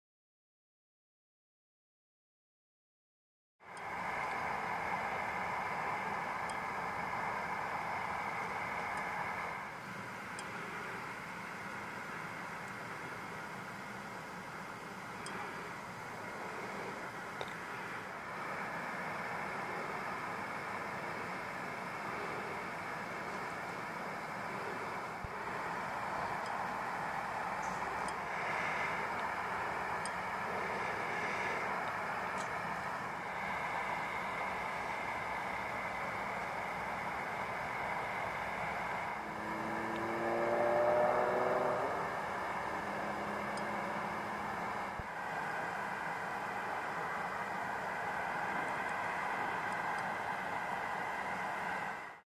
Owlets on Wimpole Estate 2013
The young of one of the three Barn Owl pairs nesting on Wimpole Estate. There are three owlets, two very well developed and the third in the tree nesting hole looking quite poor in comparison. The third owlet is a bit of a surprise as it looked as there were only two when the nest hole was inspected by Colin Shawtry
 More to come as they develop over the next few days.